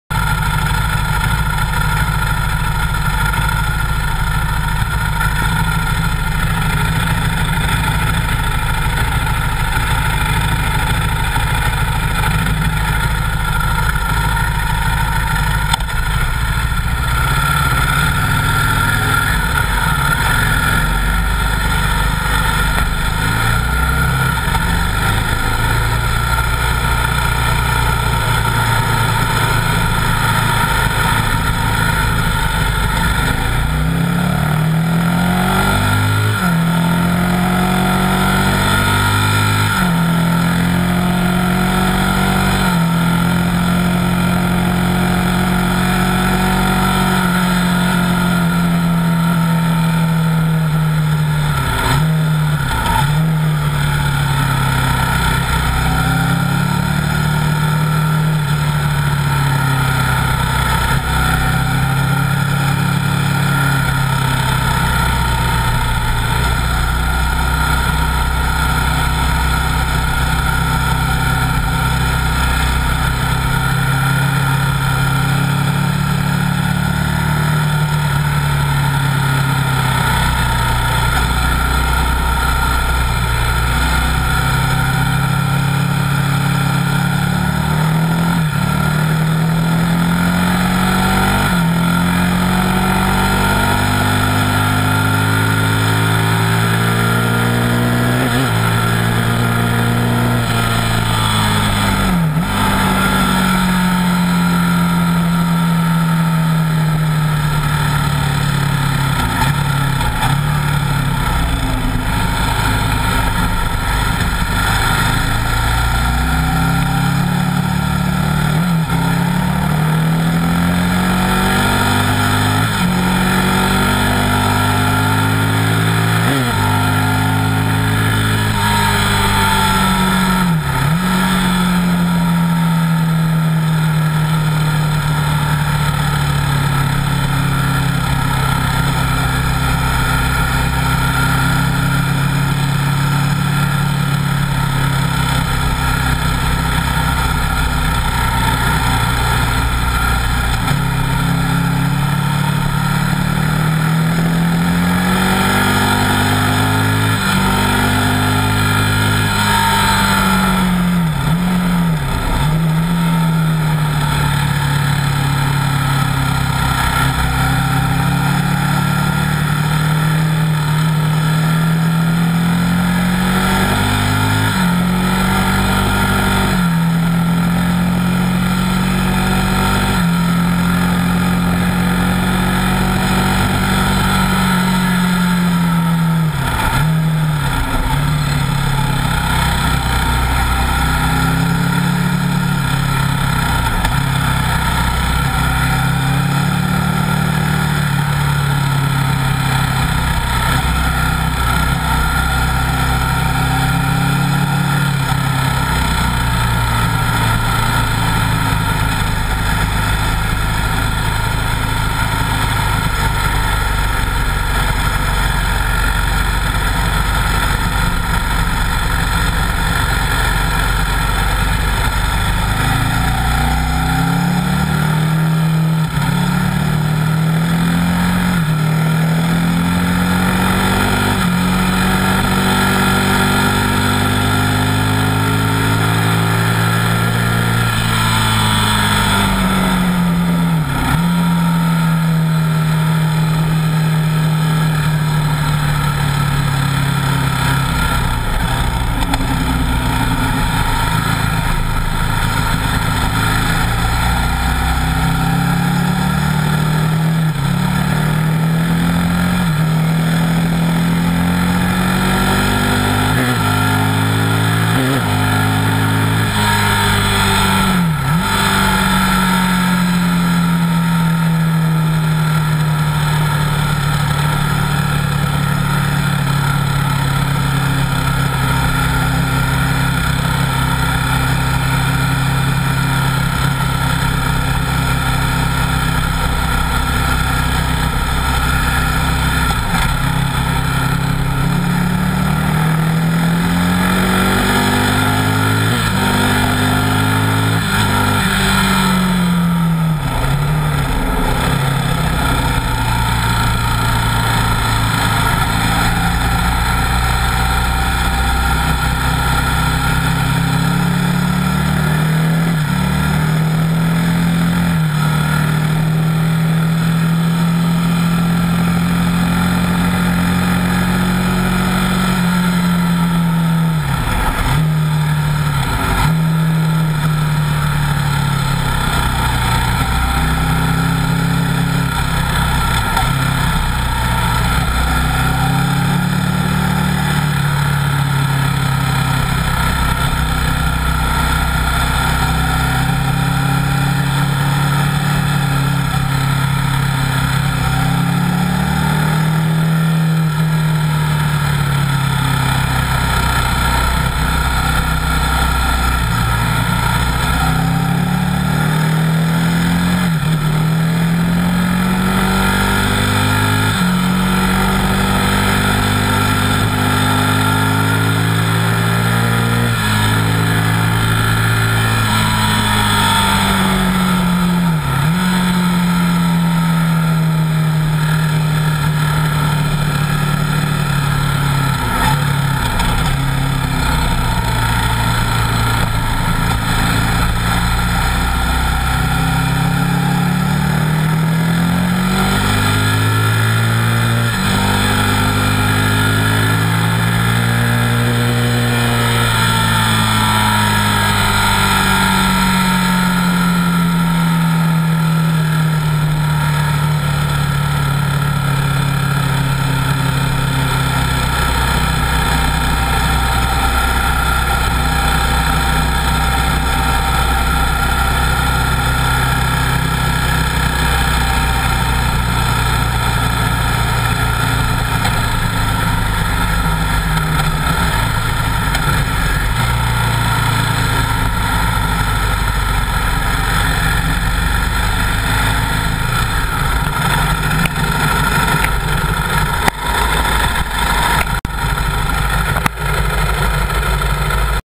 MSR 03-18-12 Afternoon Randy Semroska
Go Pro video facing rider Randy Semroska in the afternoon session there were 2 yellow flag crashes in this video. Riding a Buell 1125R on Street tires.